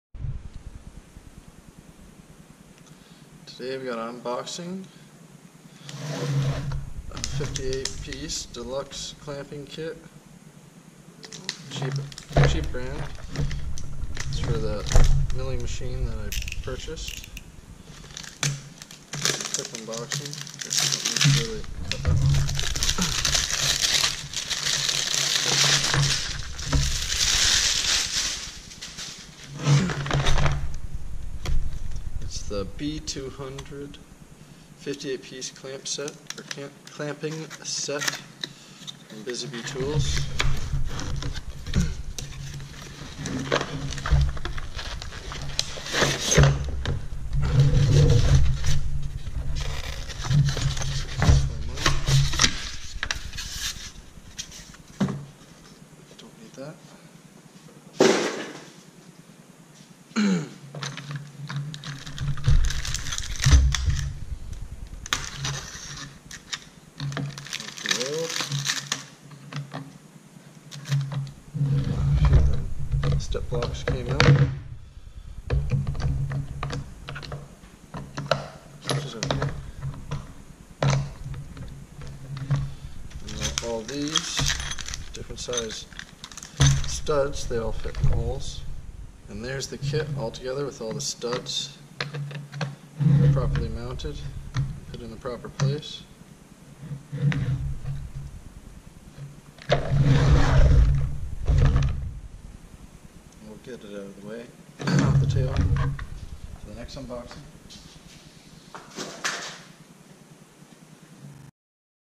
Unboxing a 1/2" T-Slot 58pcs Deluxe Steel Clamping Set
58 Piece 1/2" T-Slot Deluxe Steel Clamping Set from Busy Bee Tools, Part B200.